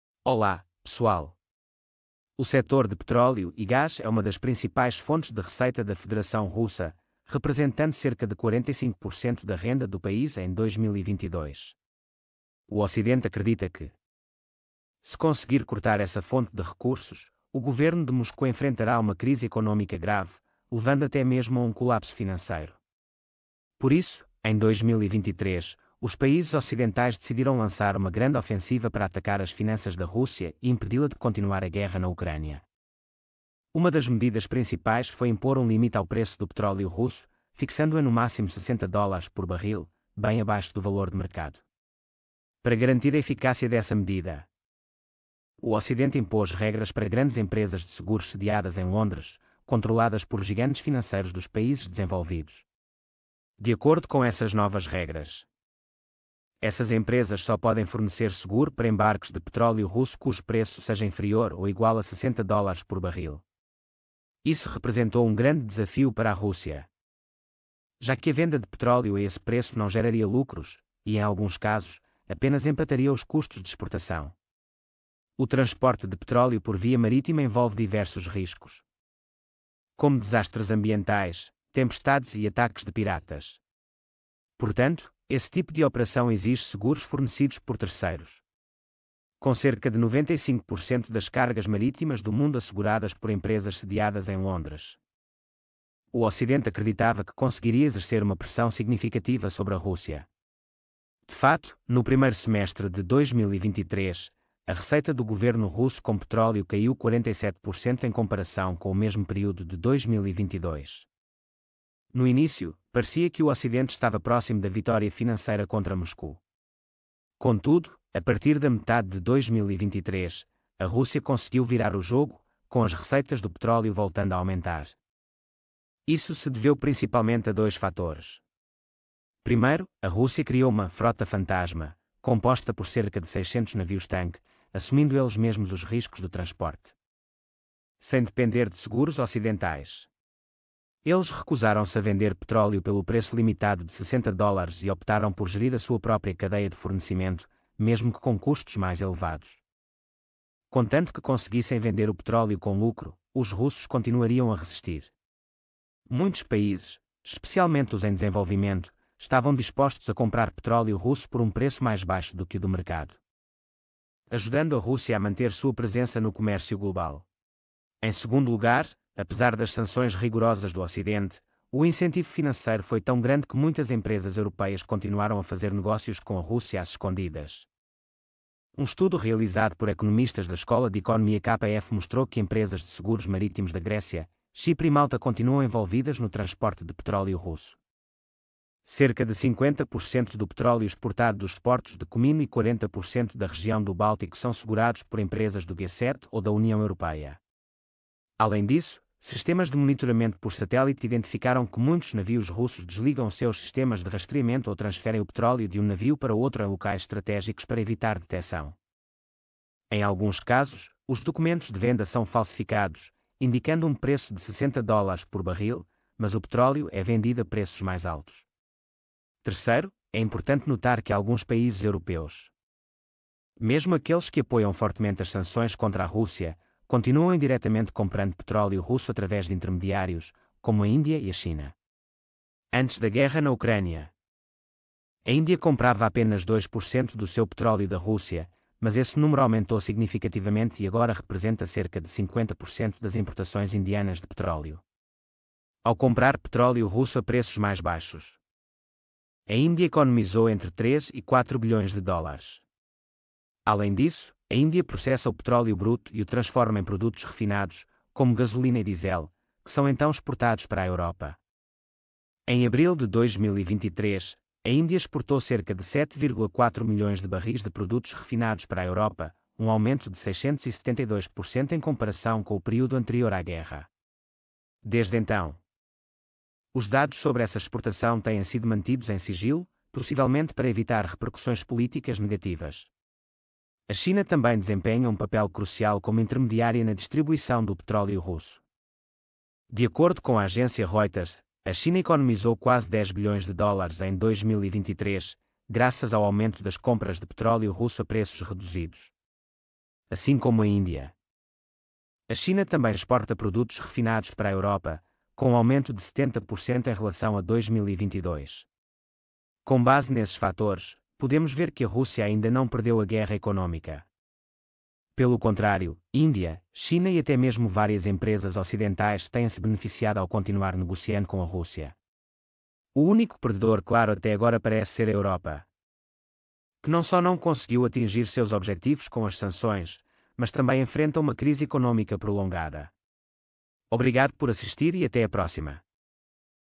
Que “cartão” a Rússia usa para superar a pressão do petróleo vinda do Ocidente?
Petróleo e gás são um dos campos que geram as maiores receitas para a Federação Russa, respondendo por cerca de 45% em 2022. Apenas cortando o fluxo de caixa a partir daqui - o governo de Moscou correrá o risco de cair no apocalipse. É por isso que: o Ocidente decidiu travar uma grande batalha em 2023 – perfurando os bolsos da Rússia, tornando impossível que continuem a travar a guerra.
A maneira simples de fazer isto é a seguinte: Limitar o preço do petróleo bruto russo a um máximo de 60 dólares/barril, muito inferior ao mercado. Para fazer isto - introduziram regulamentos para uma empresa de seguros em Londres, incluindo gigantes ocidentais. - com enorme potencial financeiro Consequentemente - este grupo só está autorizado a segurar embarques de petróleo russo com um preço máximo de 60$/barril. Contra esse preço - a Rússia atingirá o ponto de equilíbrio ou não terá lucro ao exportar. O petróleo, como muitos outros bens, é propenso a incidentes quando transportado no mar - como poluir as fontes de água dos países, tempestades ou tsunamis, ser atacado por piratas, etc., por isso precisa de garantia de terceiros/ E o grupo de Londres é responsável por cerca de 95% dos embarques mundiais por tonelagem.
Ao introduzir os regulamentos acima mencionados - o Ocidente fez com que a Rússia enfrentasse sérias dificuldades nos primeiros 6 meses de 2023. Especificamente - as receitas petrolíferas do governo de Moscovo diminuíram até 47% em comparação com o mesmo período de 2022. Ao vencer, parecia que estava ao alcance do Ocidente - mas nos meses seguintes, até agora, a Rússia subiu a montante - com as receitas do petróleo a aumentar continuamente, quando os preços de exportação estão próximos dos preços de mercado. O que aconteceu?